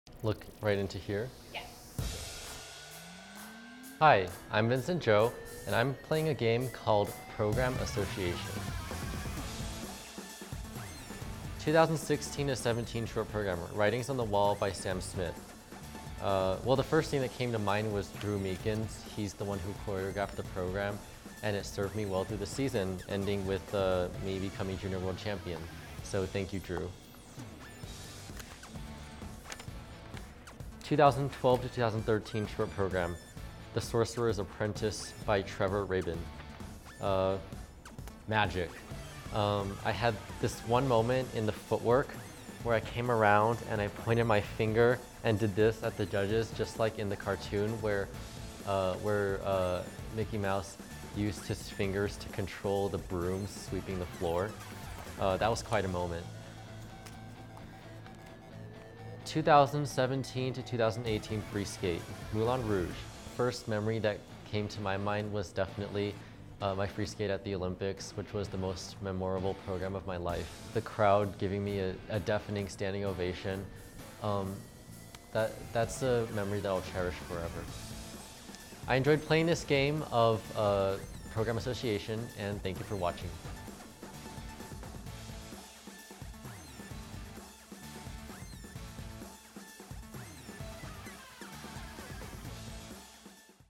Vincent Zhou Plays Program Association | U.S. Figure Skating
Olympian Vincent Zhou is playing Program Association! From his World Junior title to the Olympic Winter Games PyeongChang 2018, he shares a few stories behind his programs.